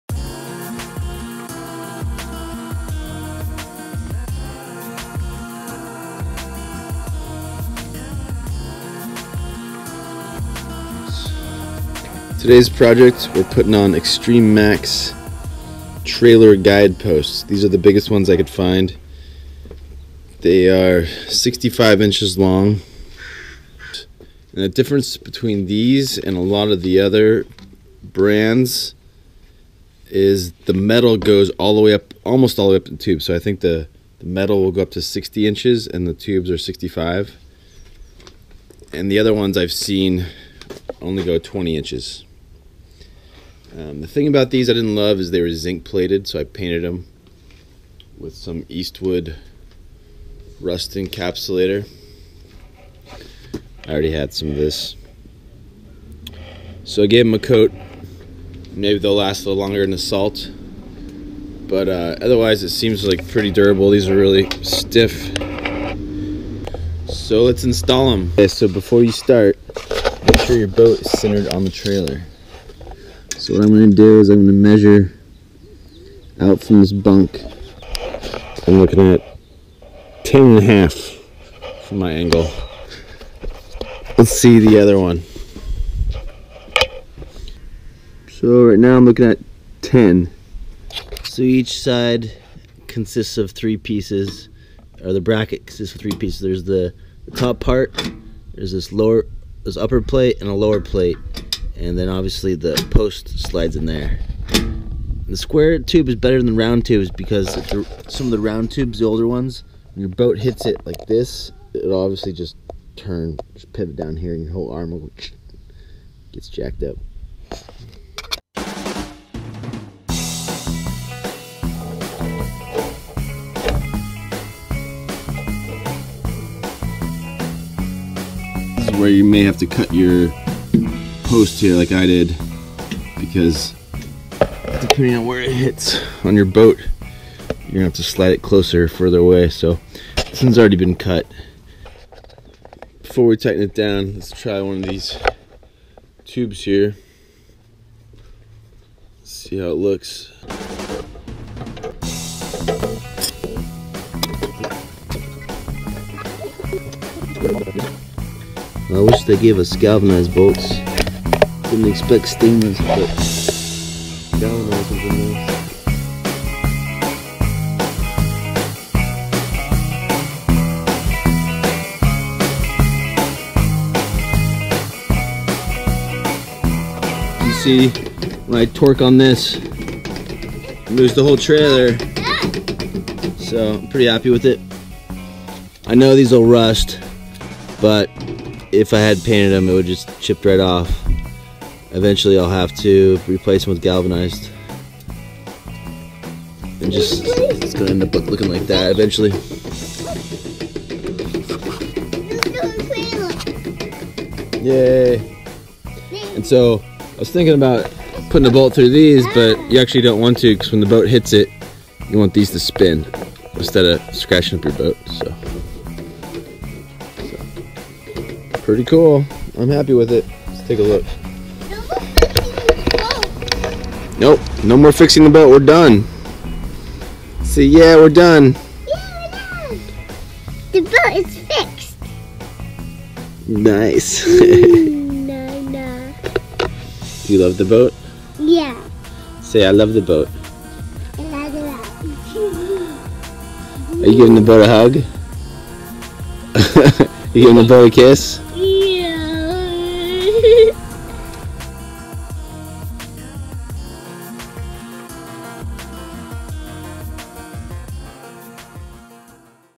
Install Boat Trailer Guides to Keep Your Boat Straight When Loading
Extreme Max Trailer Guide on Posts 65"

How to make loading your boat on your trailer so much easier and faster. My boat would rarely be straight on my bunk boat trailer when I pulled it out of the water. These trailer guides fixed it.